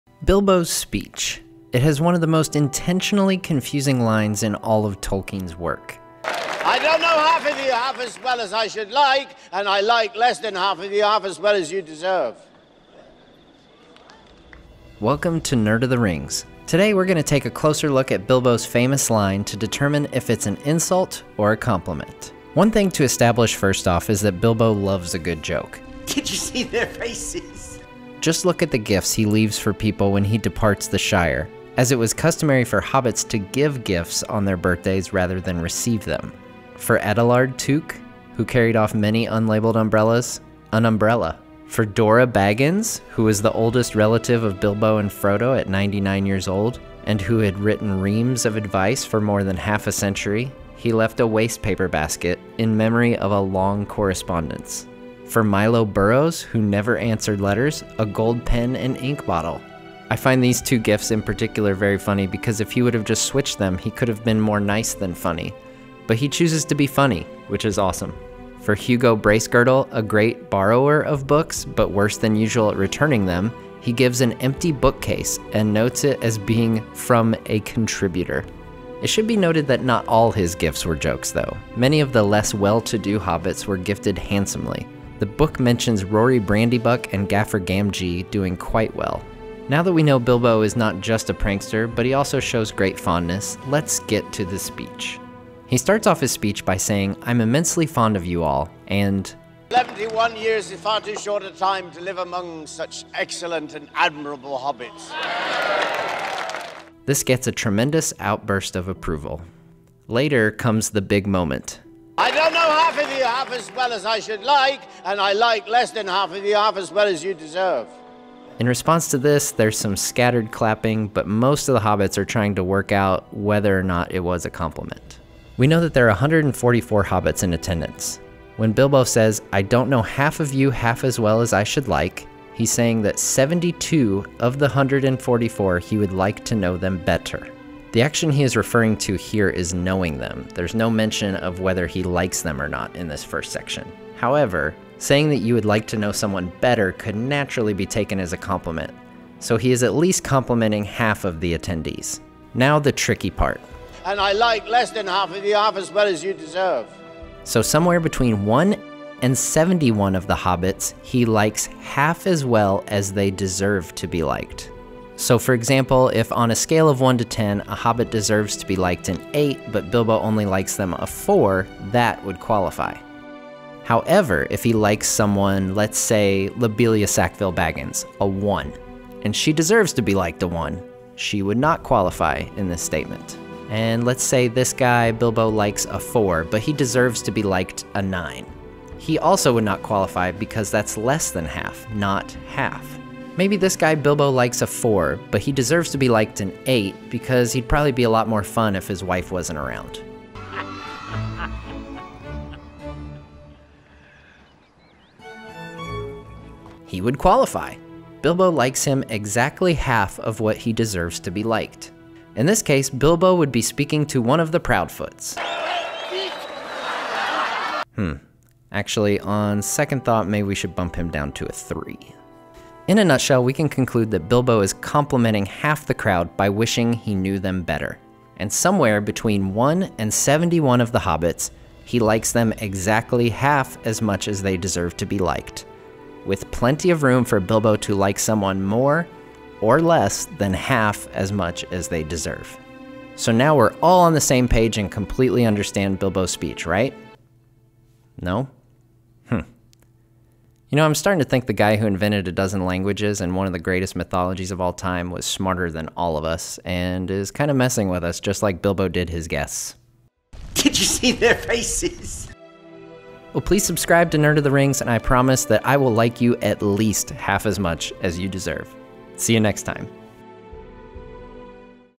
Bilbo's Speech: Insult or Compliment? | Tolkien Explained
Is Bilbo's famous line an insult or a compliment? Yes! (lol.) Basically, it's a bit of both! In the video, I break down why!

"I don't know half of you half as well as I should like, and I like less than half of you half as well as you deserve!"  It's one of my favorite lines from Fellowship of the Ring. It's clever, it's complimentary (to some) and insulting (to others), and it shows how our lovable burglar, Bilbo Baggins loves a good joke!